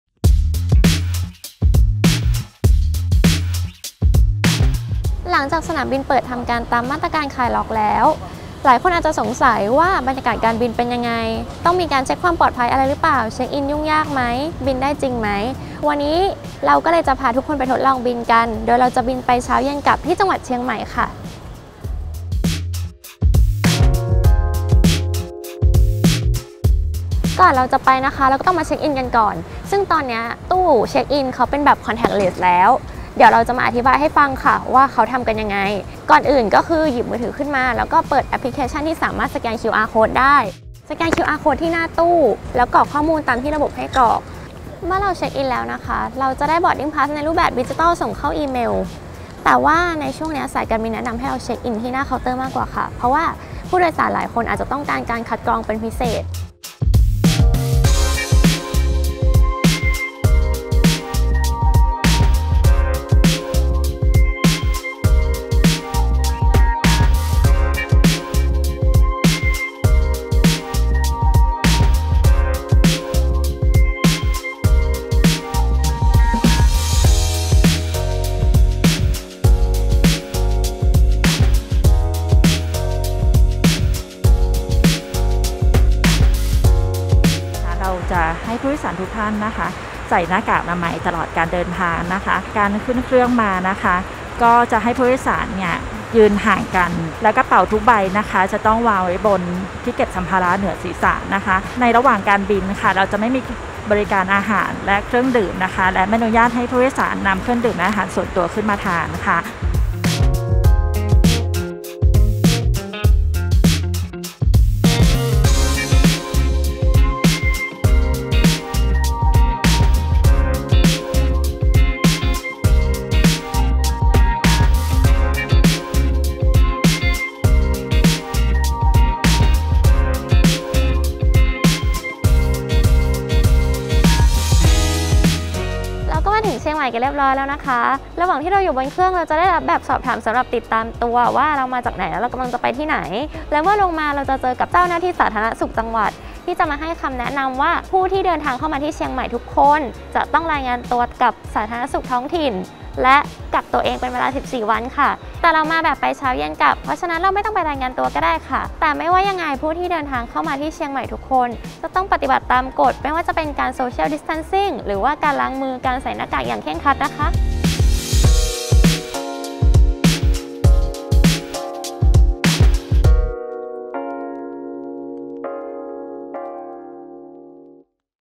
ทดลองนั่งเครื่องบินยุค New Normal ปลอดภัยขนาดไหน?
หลังจากที่วันก่อนเราเผยบรรยากาศการบินในรูปแบบของภาพนิ่ง วันนี้ THE STANDARD POP ขอพาไปชมอีกเวอร์ชันที่เป็นภาพเคลื่อนไหวว่าบรรยากาศการบินในยุคหลังโควิด-19 มีหน้าตาเป็นอย่างไร ปลอดภัยขนาดไหน และมีกฎระเบียบยุ่งยากเพียงใด กับไฟลต์บินแบบไปเช้าเย็นกลับ เส้นทางกรุงเทพฯ-เชียงใหม่-กรุงเทพฯ